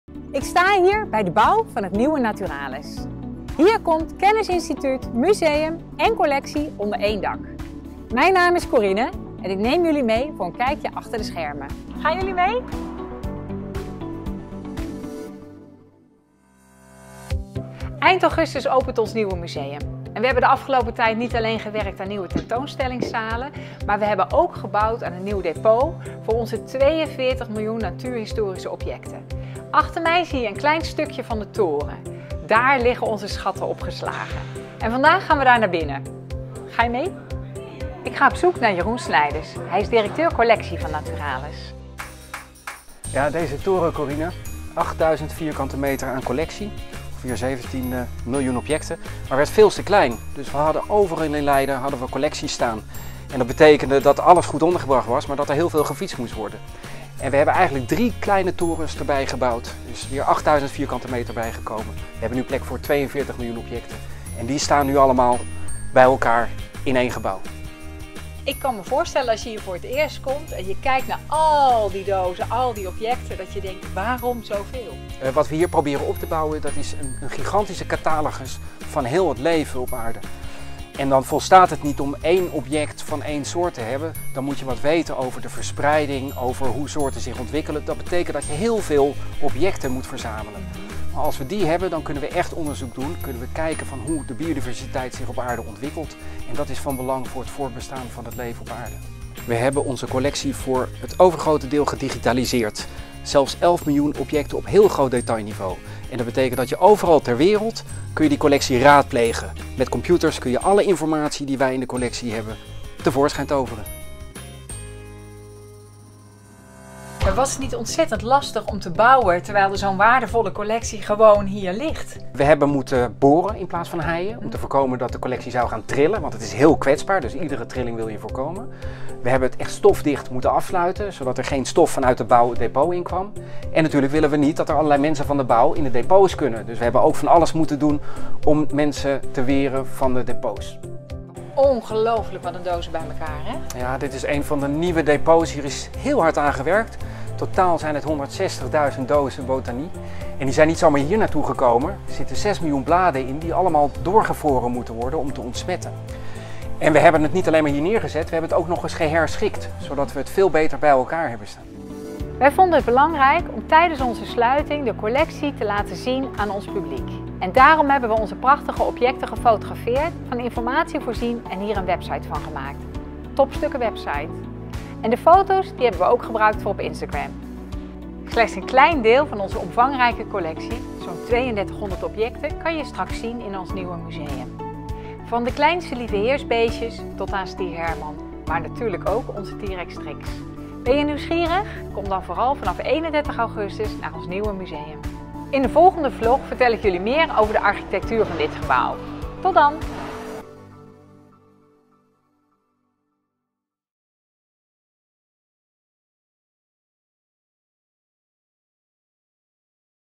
Naturalis bouwt - Vlog 3: de collectie
Naturalis verbouwt. Medio 2019 wordt de nieuwbouw geopend. Niet alleen een museum, maar een kennisinstituut waar álles bij elkaar komt. Corine van Impelen is hoofd communicatie bij Naturalis en geeft je een kijkje achter de schermen via een reeks vlogs. Hoe ver zijn ze met de bouw?

In deze derde aflevering neemt Corine een kijkje in de nieuwe depotruimtes, waar ruimte is voor 42 miljoen natuurhistorische objecten (!). Samen met Jeroen Snijders, directeur collectie, duikt ze in de collectie en ontdekt ze wat er nodig was om deze schatten ongeschonden door de verbouwing te loodsen.

Mede mogelijk gemaakt door Naturalis en de Museumvereniging.

Meer informatie en inspiratie?